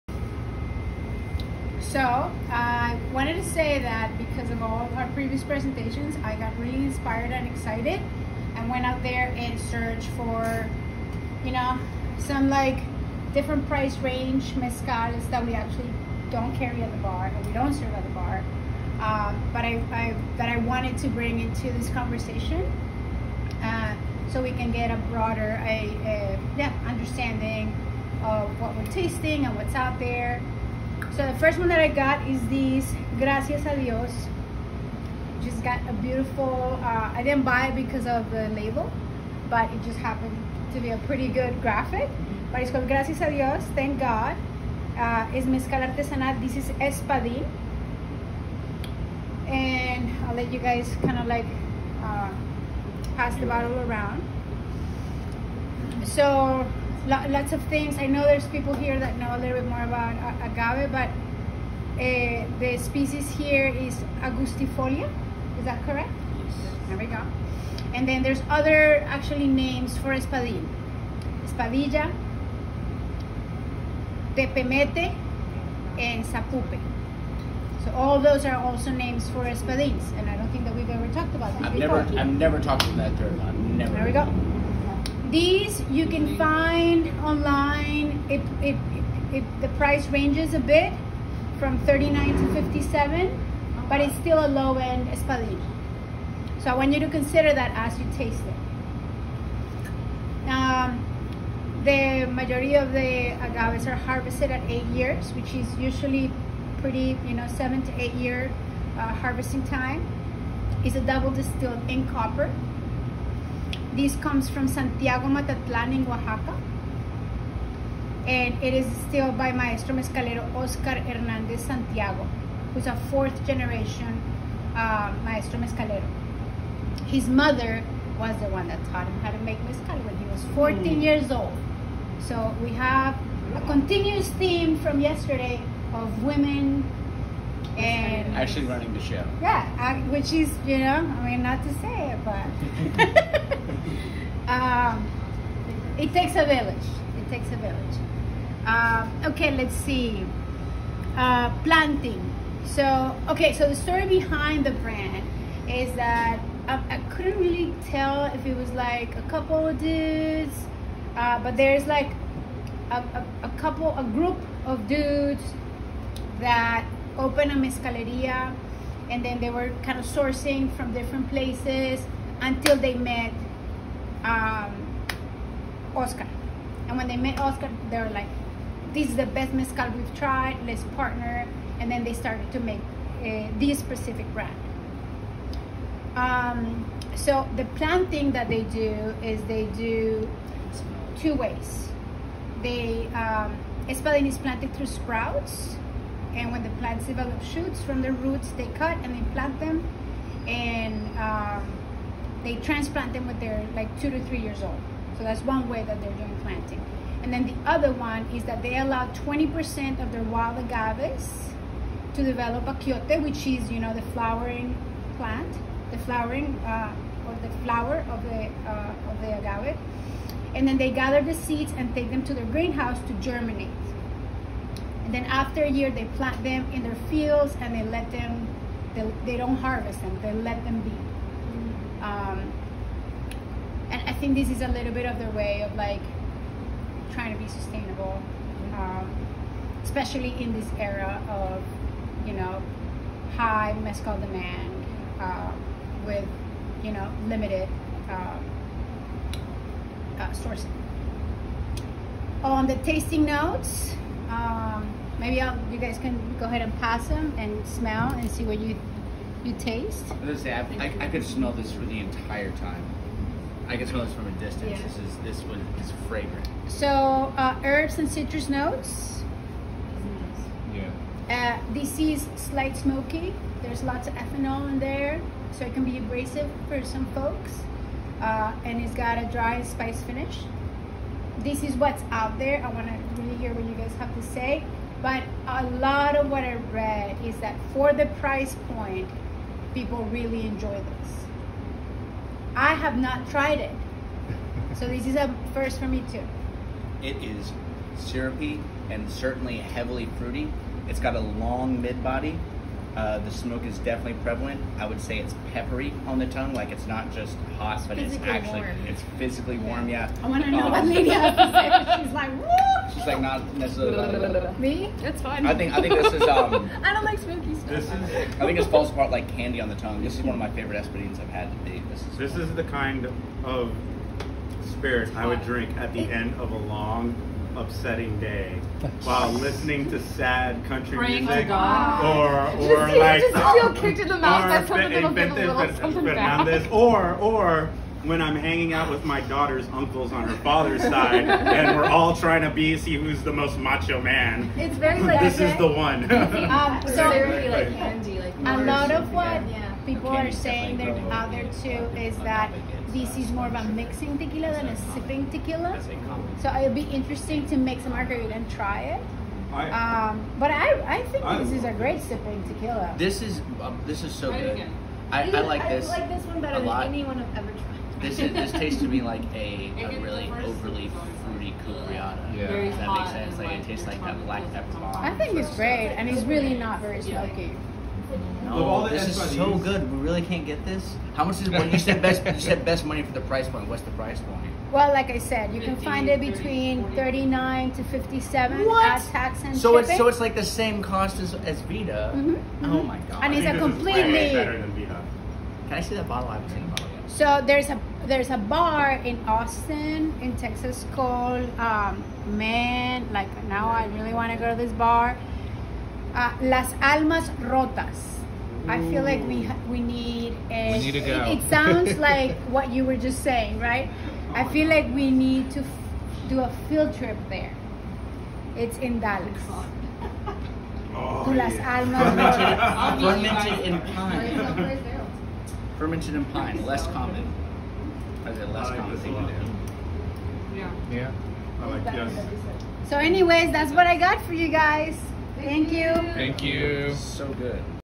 Gracias a Dios Espadin Jove
Angela Salamanca presenting Gracias a Dios Espadin Joven to the team.